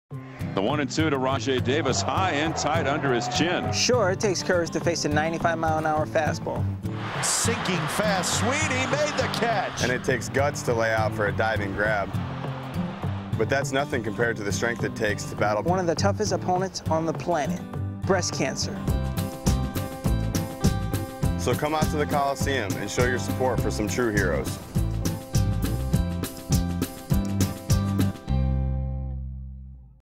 Breast Cancer Awareness Day Oakland A's - Music From Musync
Pre-Cleared Music from Musync for Oakland A's Breast Cancer Awareness Day